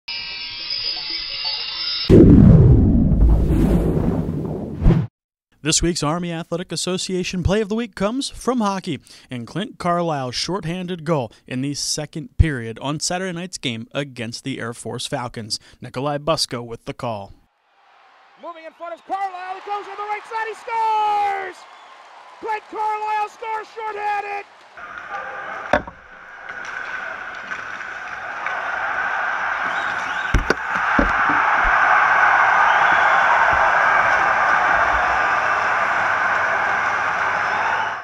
Play of the Week: 1-13-14
This week's Army Athletic Association Play of the Week comes from the hockey team. Clint Carlisle's short-handed goal to give the Black Knights a 2-0 lead over rival Air Force on Saturday night.